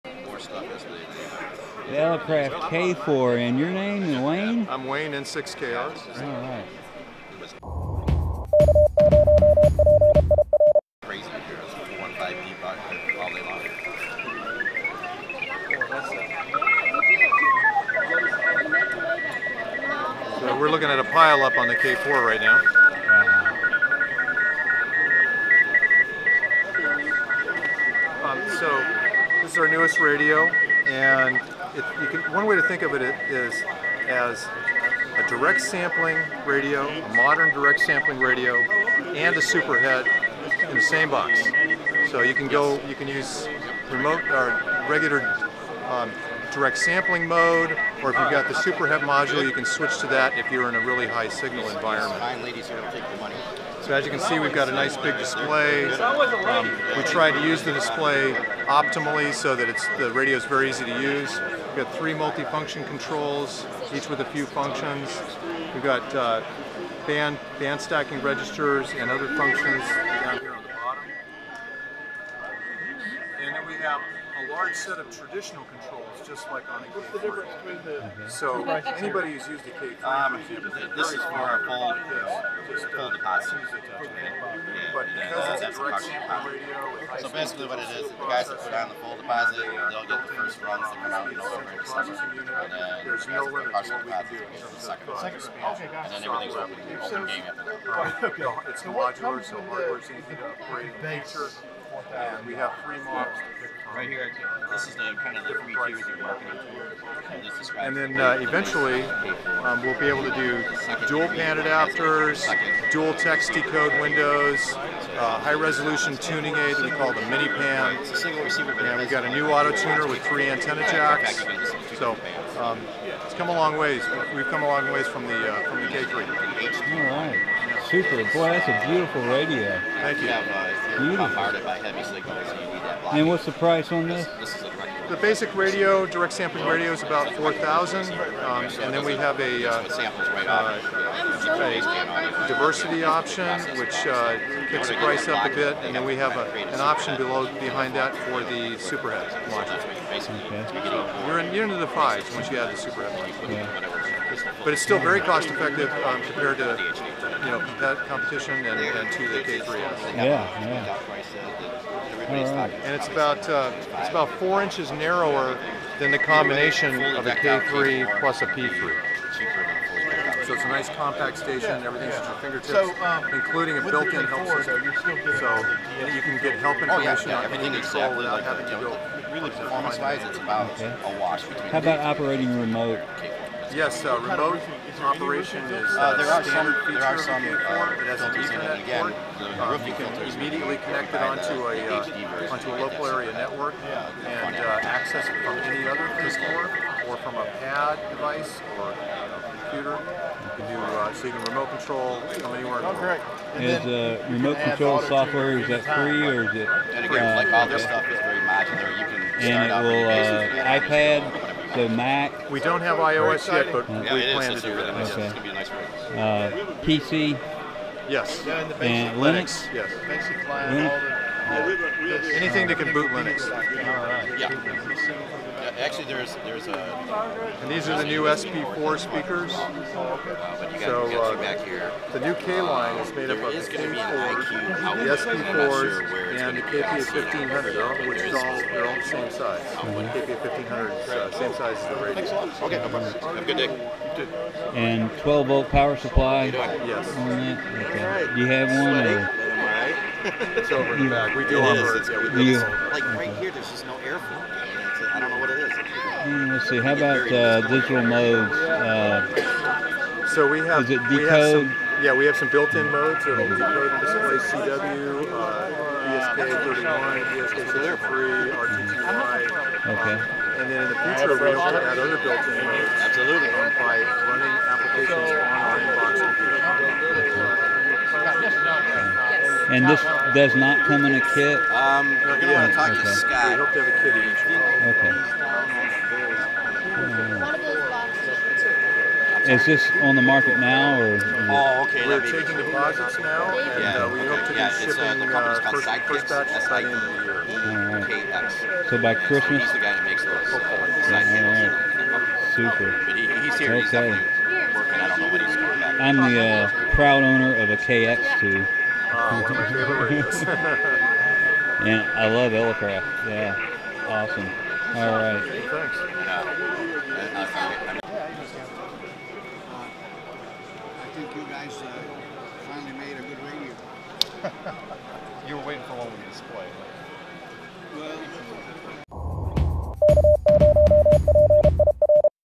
NEW Elecraft K4 K-Line at Hamvention
We’re pleased to announce our new top of the line transceiver, the Elecraft K4. It combines the latest in direct-sampling technology with classic controls and an upgradeable, modular architecture. You’ll find a full Introduction below, as well as technical and ordering information.

We think you’ll find the K4 to be the advanced radio you’ve been waiting for...powerful and easy to use, backed by an engineering team that loves to get on the air.

73,

Wayne, N6KR

Eric, WA6HHQ

○○○ LINKS ○○○